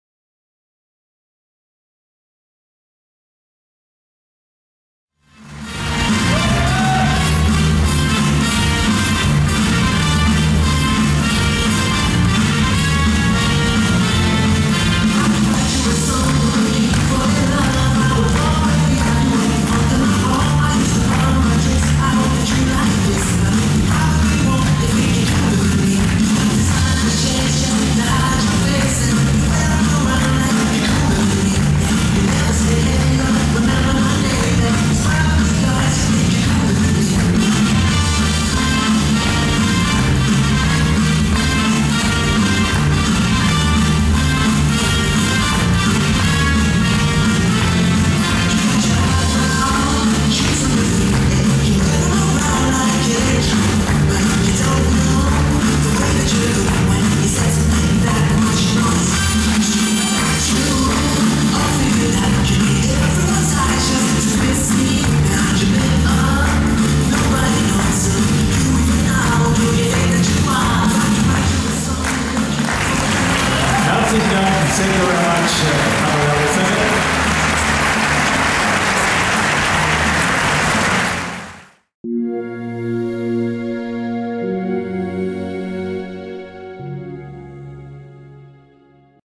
Kagan - Selivanova, EST | 2015 PD World LAT | solo J
Saxonian Dance Classics in Dresden, Germany - 2015 WDSF PD World Latin